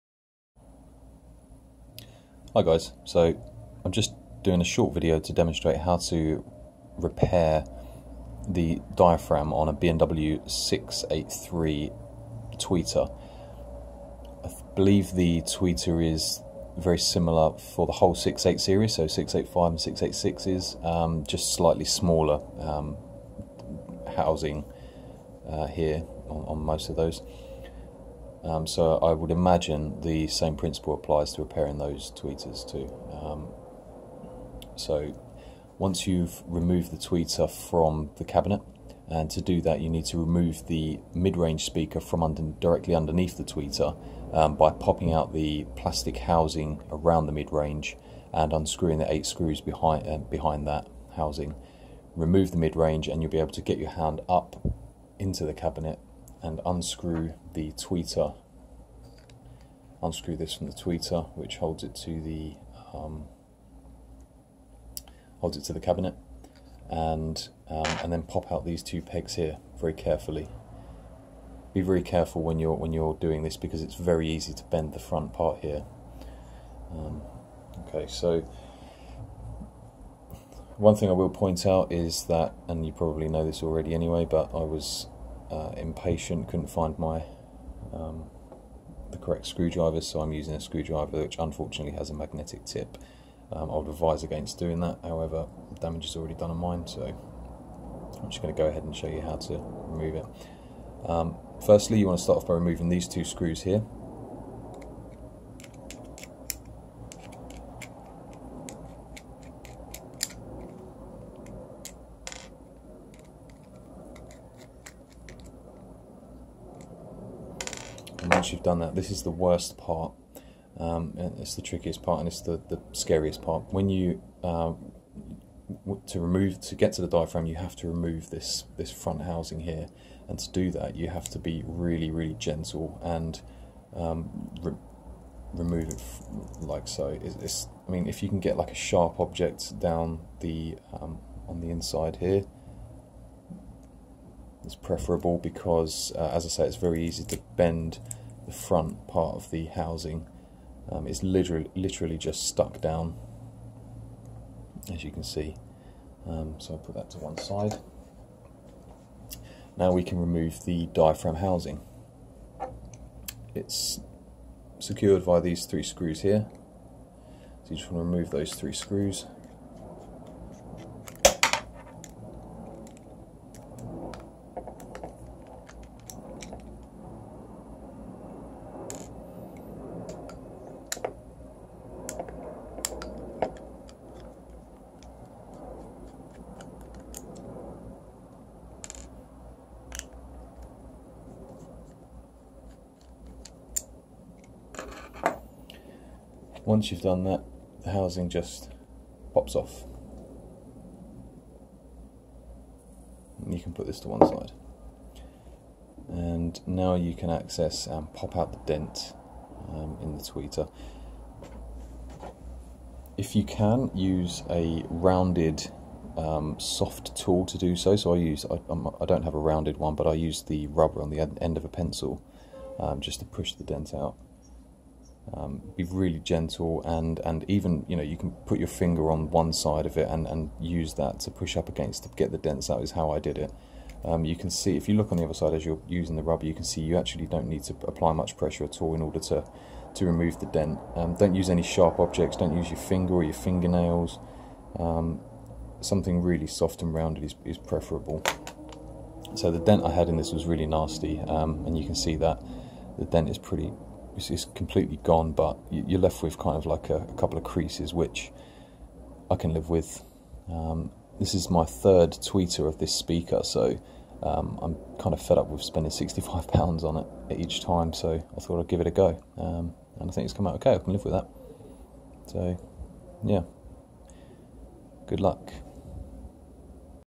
Bowers and Wilkins 683 tweeter dent repair
A quick demonstration on how to disassemble the tweeter housing to push out dents in the tweeter diaphragm